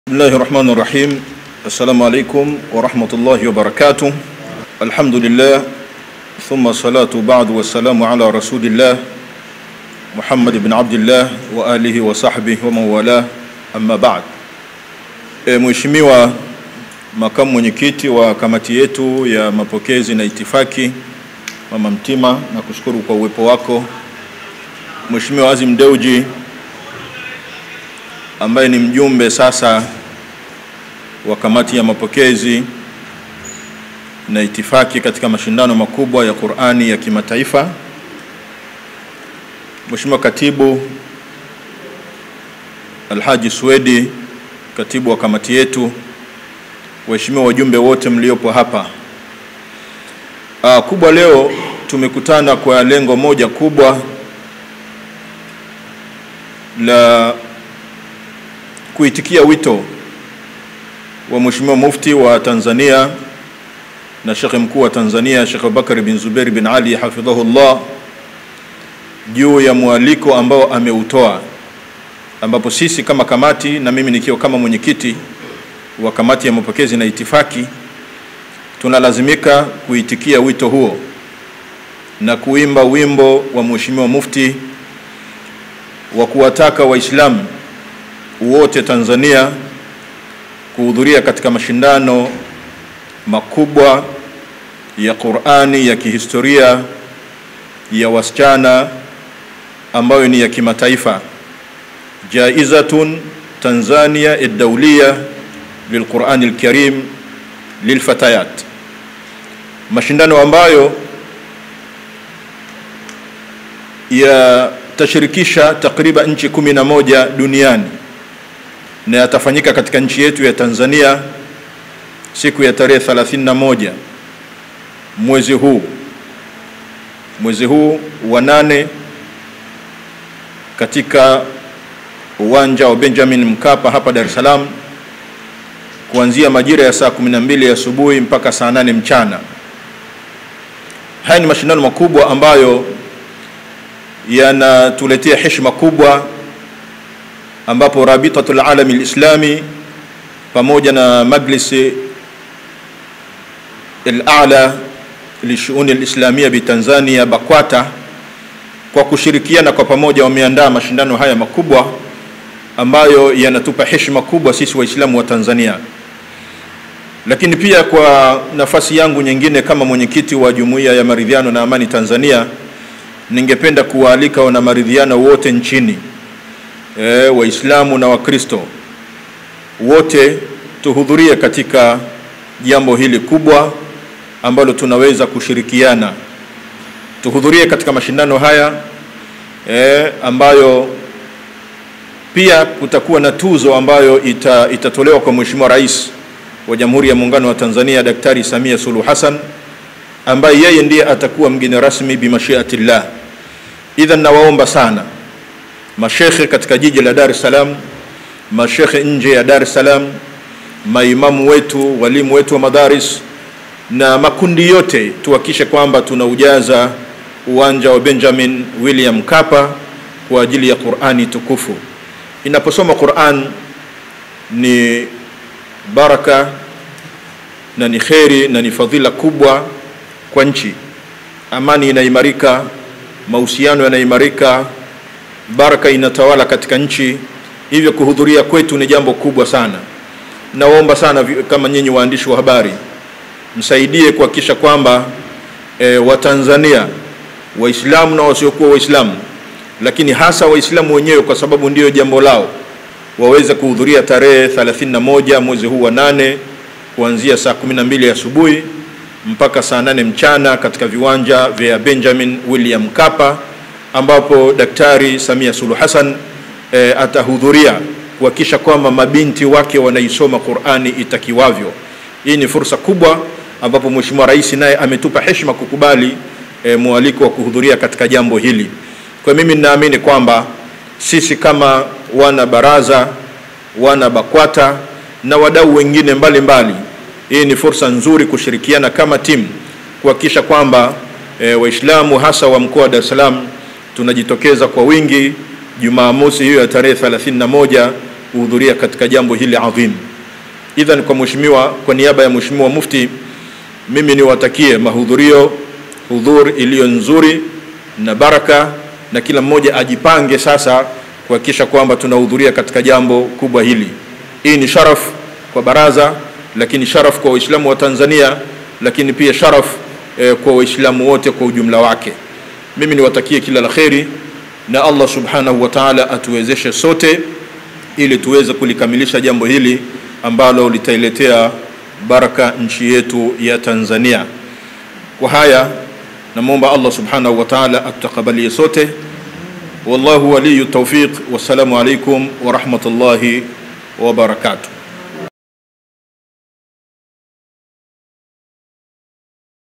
Njooni Kwa Wingi Kuja Kuangalia Mashindano Ya Wanawake Ya Qur-an Yatakayofanyika Ktk Uwanja Wa Mkapa
Mwenyekiti wa kamati ya Amani na Maridhiano Tanzania (Jmat) awahimiza waislamu wote kujitokeza kwa wingi Uwanja Wa Mkapa katika Mashindano ya Quran mgeni rasmi akiwa ni Rais Samia Suluhu Hassan tarehe 31/2024